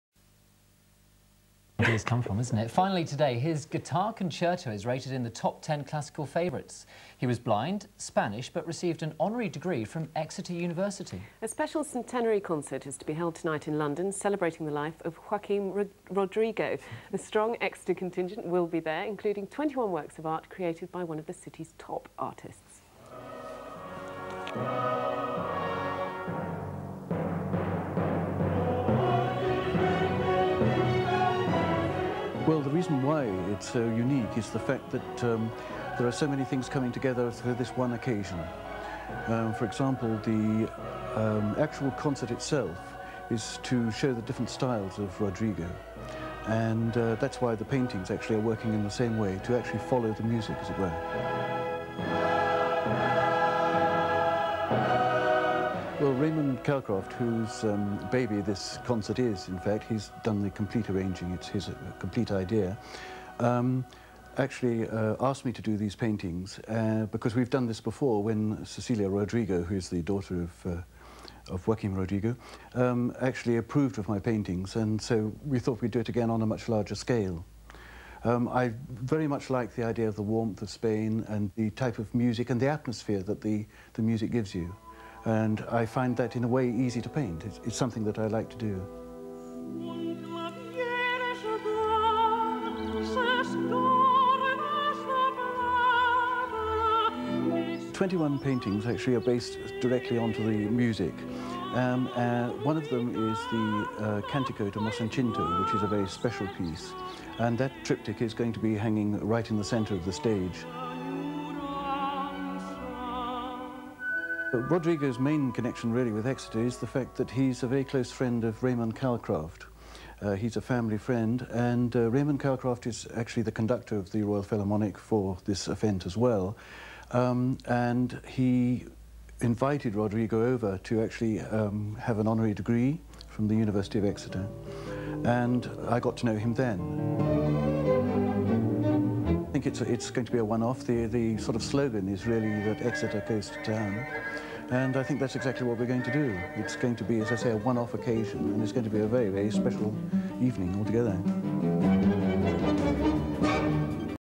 Interview with Nicholas Eastwood: Joaquin Rodrigo Celebration concert paintings
A Rodrigo Celebration concert paintings by Nicholas Eastwood